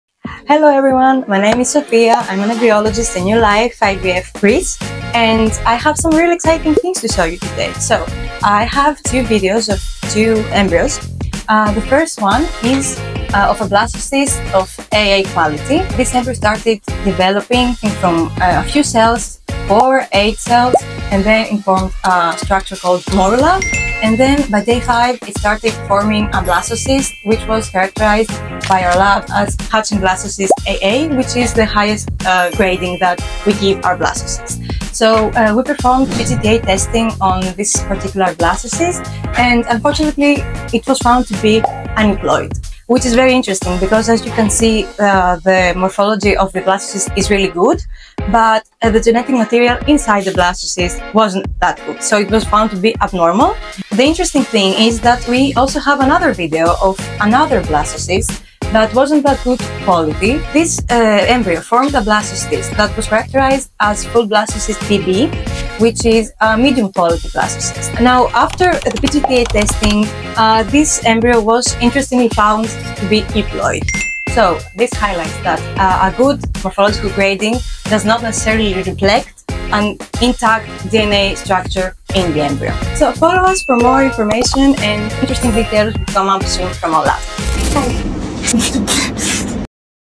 Euploid Embryo Vs Aneuploid Embryo
Does morphological grading guarantee good genetic material in the embryo?

One of our embryologists, Sophia, is here to share some valuable insights!
Follow us for more embryological tips and stay updated on the latest in fertility science.